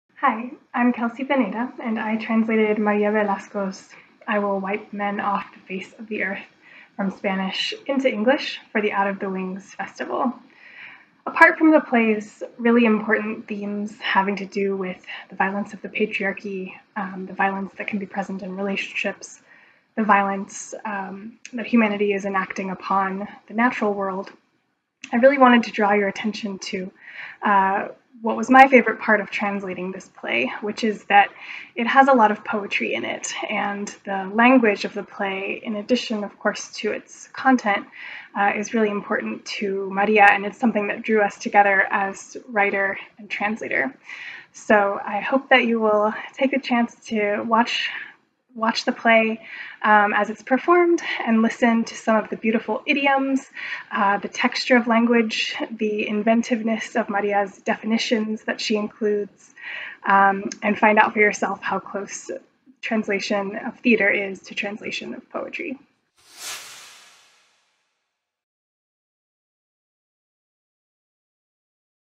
OOTW2022 Translator Kelsi Vanada comments on "I will wipe men off the face of the earth"
Translator Kelsi Vanada comments on "I will wipe men off the face of the earth" by Spanish author María Velasco, which is part of our Festival 2022.